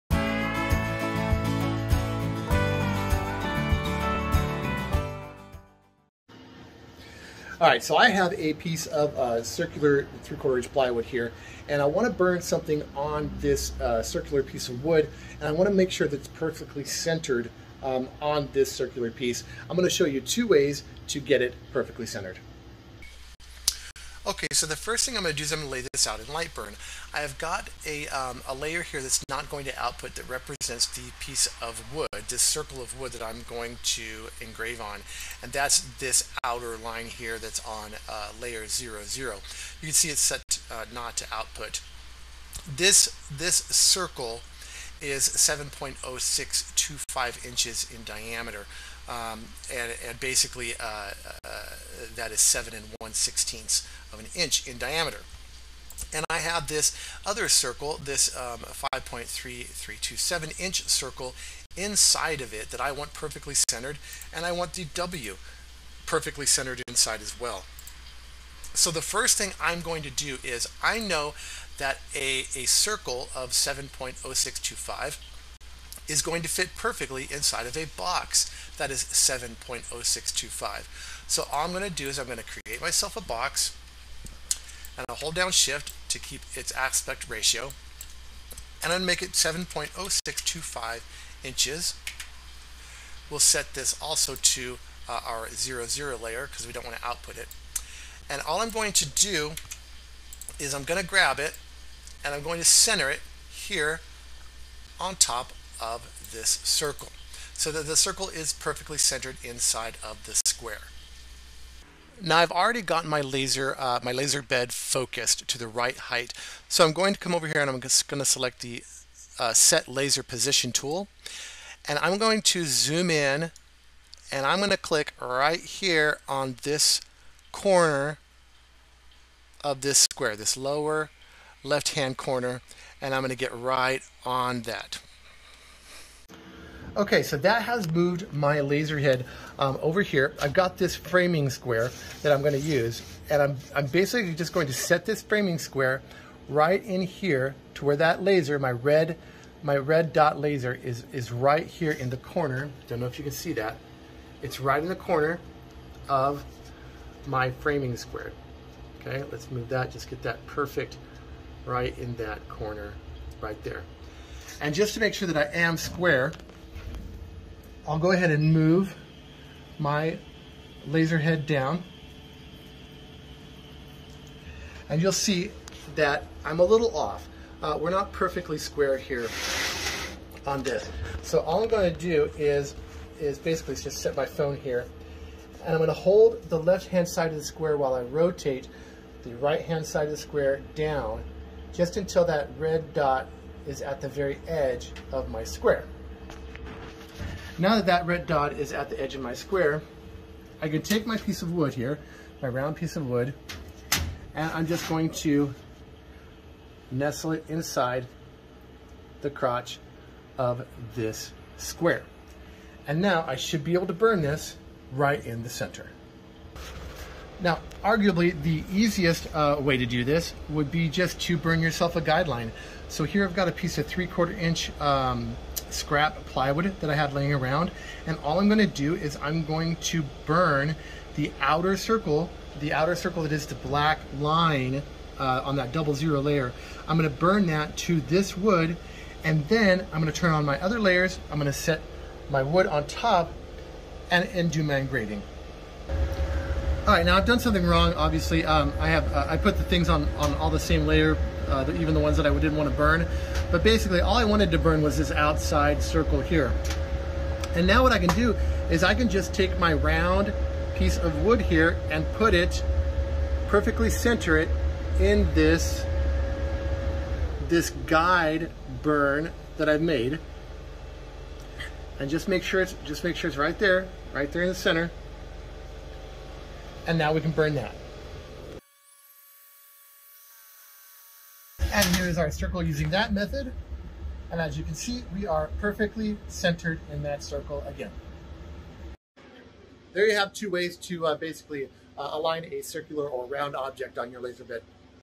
Align a round object to perfectly center your laser engraving
I show you two ways I use to perfectly align circular objects for laser engraving on my Thunder Nova 51/130